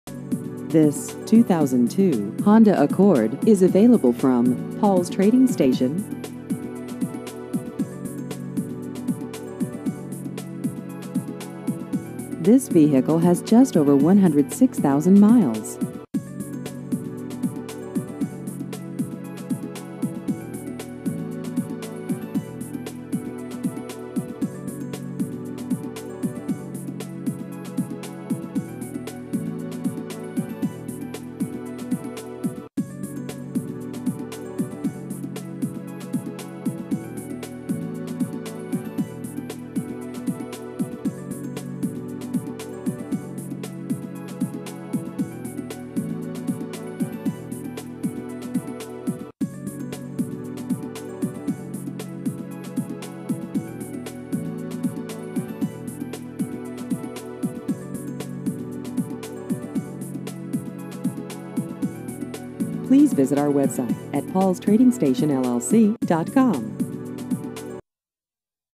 2002 Honda Accord Used Cars Mount Juliet TN
This 2002 Honda Accord is available from Paul's Trading Station LLC.